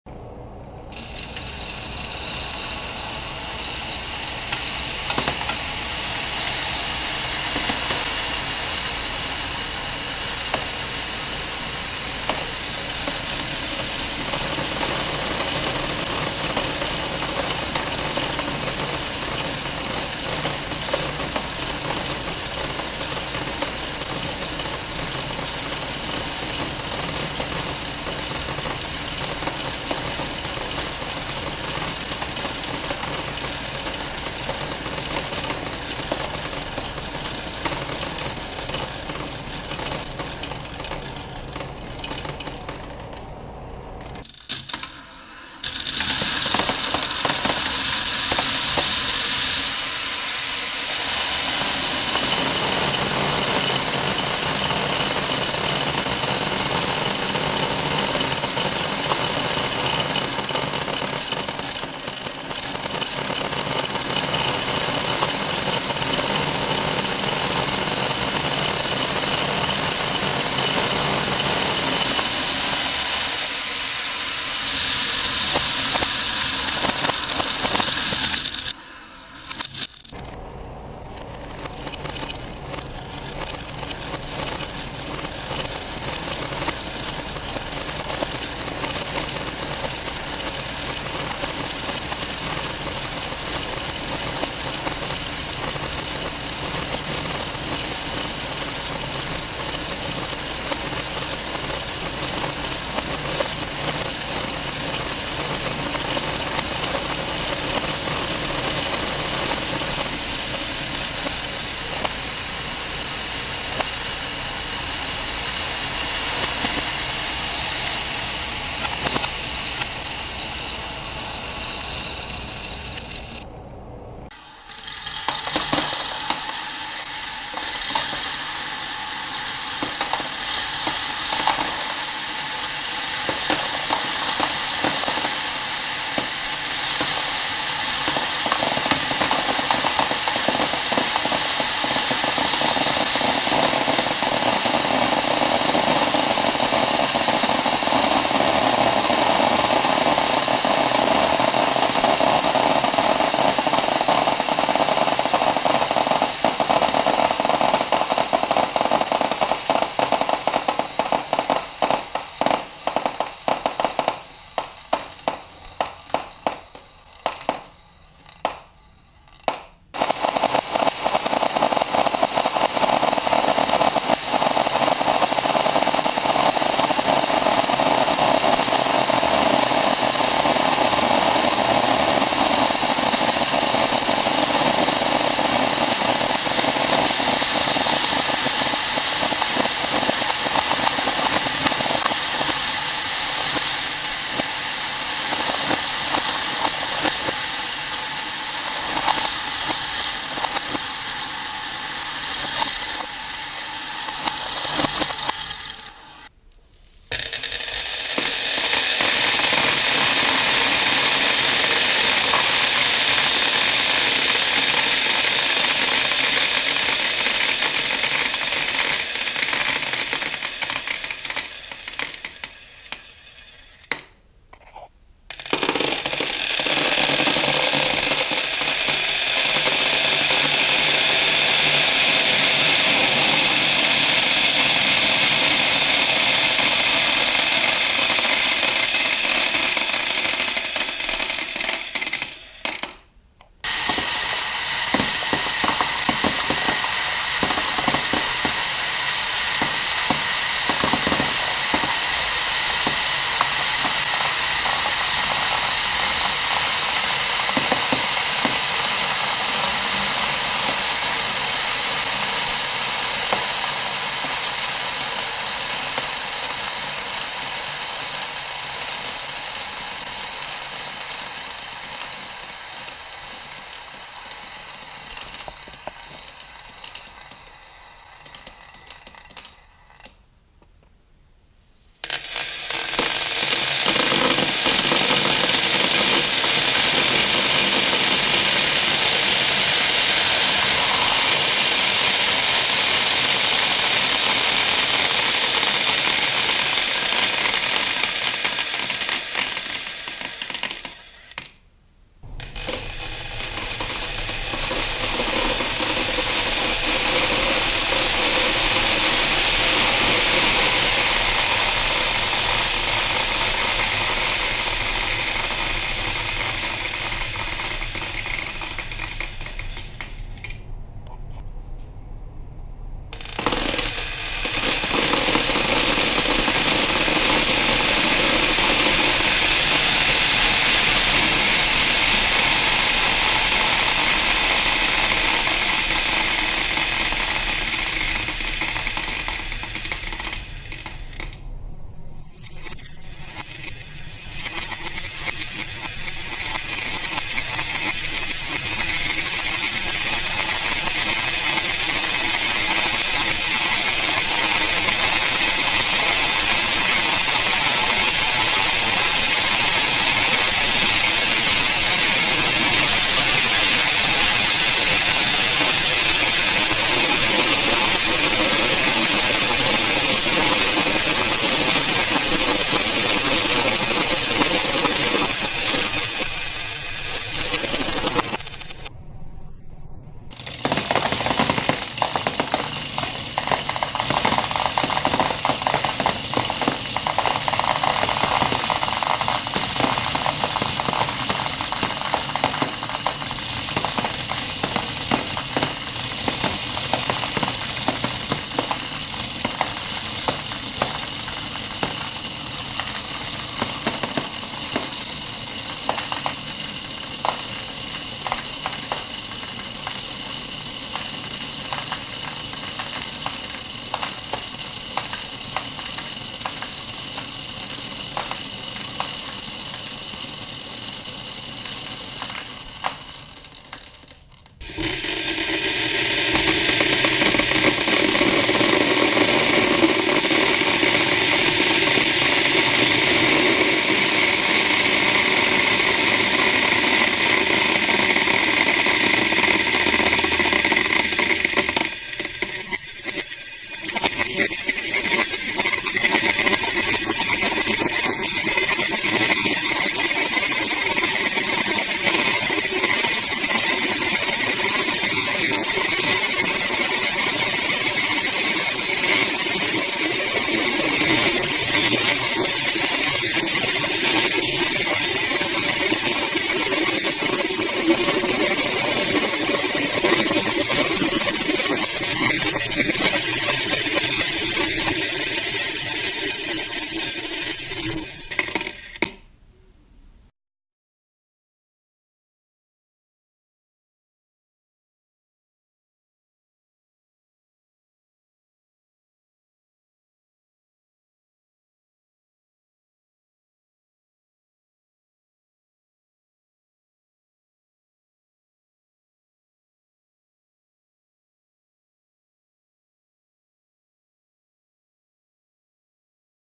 ASMR 6000 ball bearings in vortex funnels- Satisfying!!
These ASMR sounds of ball bearings rolling is very satisfying!! These are actually BB's but it's the same thing.